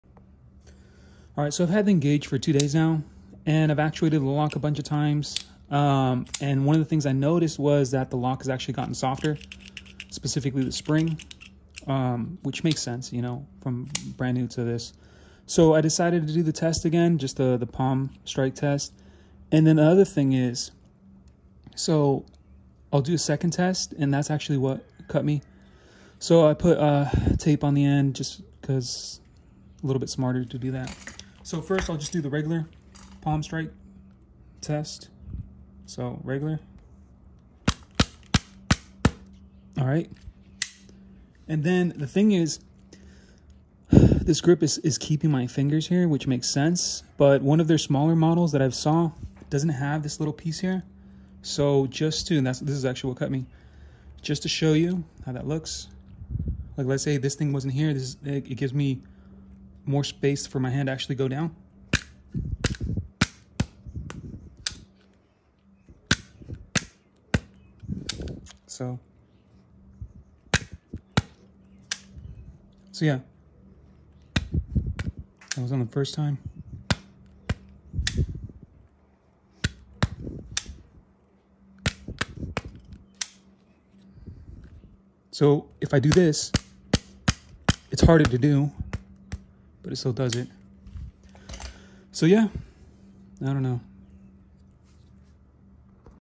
Cold Steel Engage Atlas lock fail in butt strike test
I noticed that the spring for the Cold Steel Atlas lock got softer to actuate so I decided to test it again.

The way the Atlas lock fails this test is because the lock is in my palm and because what is needed to actuate the lock is for it’s tab to be pulled towards the butt of the knife. So when I do a butt strike: the strike causes my hand to slide down which then actuates the lock.